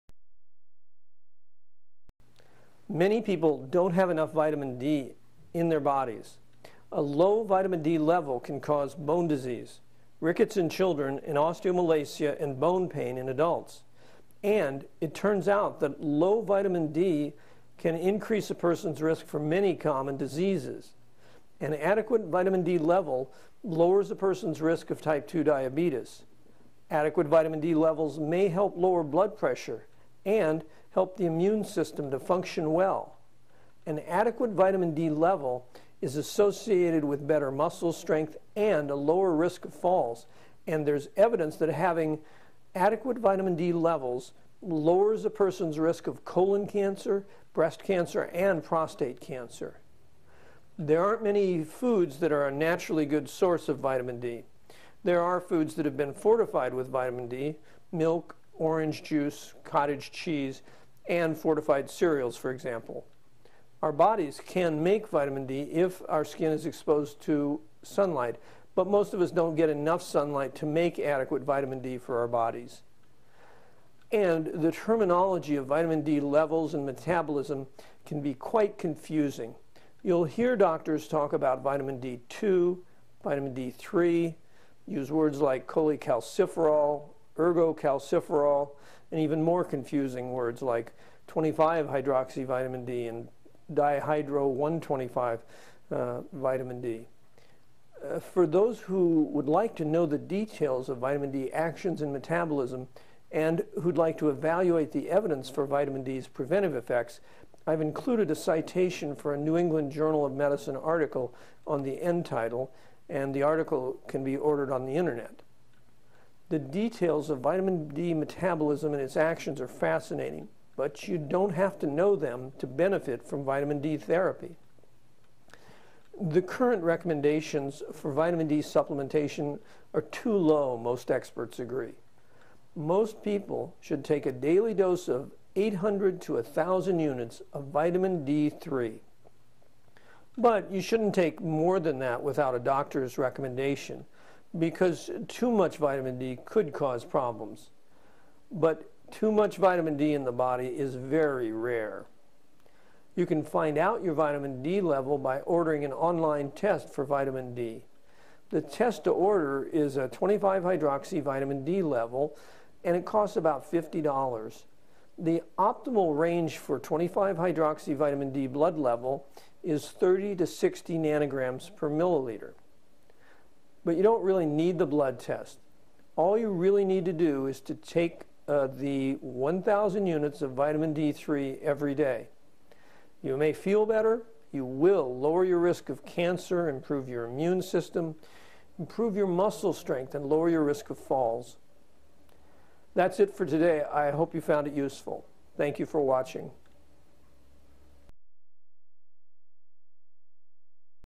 Vitamin D For Prevention
Taking Vitamin D can lower your risk of osteopenia, cancer, and falls.